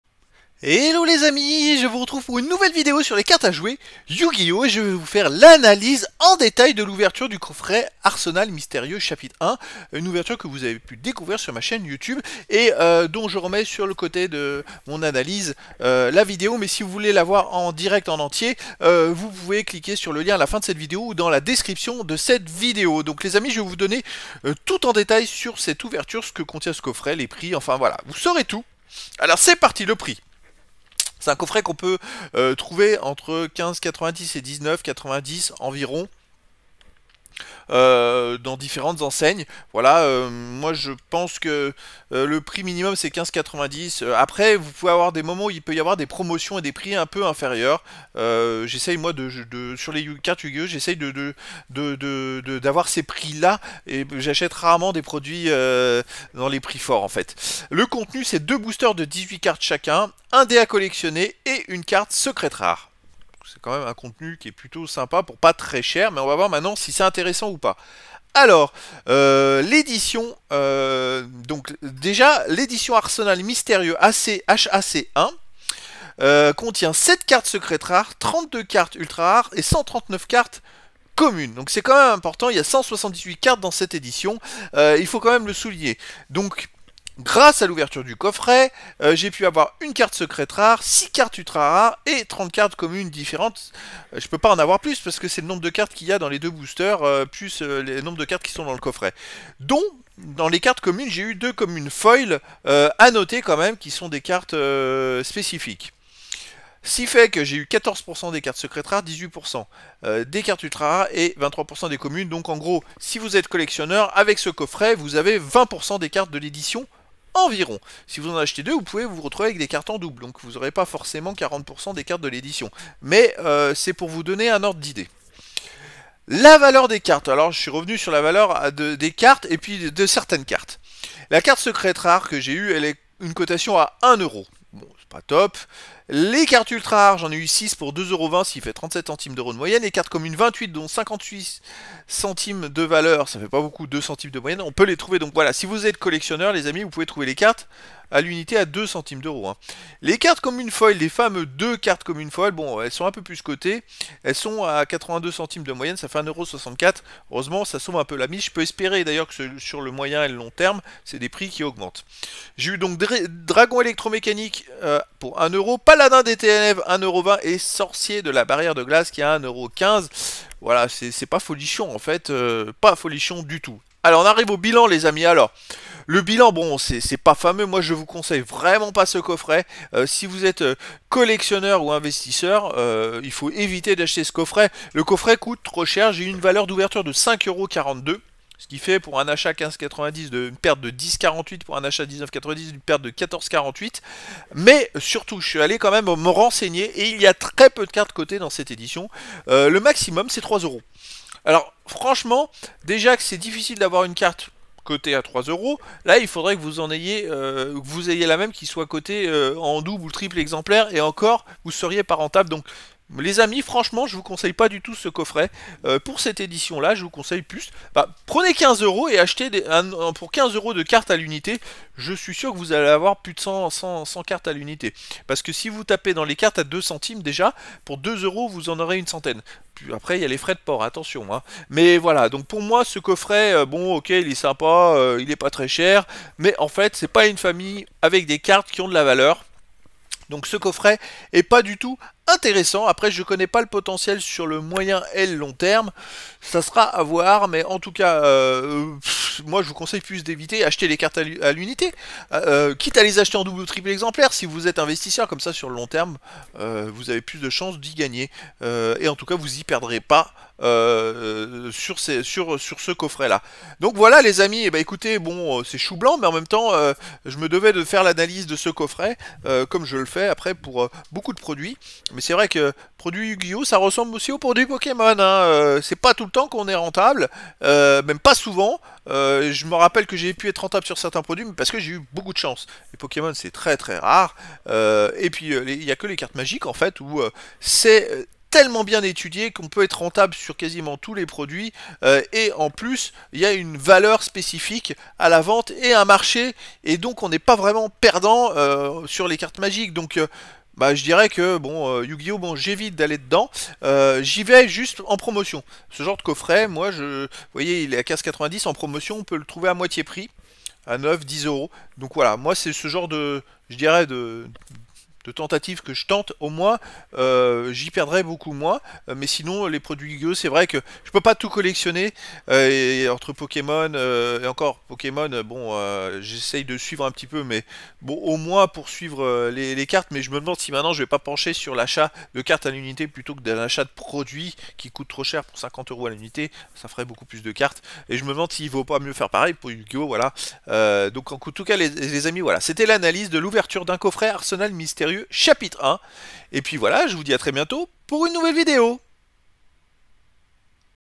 Stats et cotations de l'ouverture du coffret Yugioh Arsenal Mystérieux Chapitre 1
Je vous propose une analyse en détail de l'ouverture du coffret Yugioh de l'édition Arsenal Mystérieux Chapitre 1 (HAC1).

Je vous donne tous les prix, le contenu de ce coffret, le nombre de cartes différentes obtenues et également la cotation des cartes et la rentabilité du coffret.

J'en profite pour vous communiquer un maximum d'informations et de conseils.

Vous pouvez trouver la vidéo de l'ouverture de ce coffret au lien ci-dessous :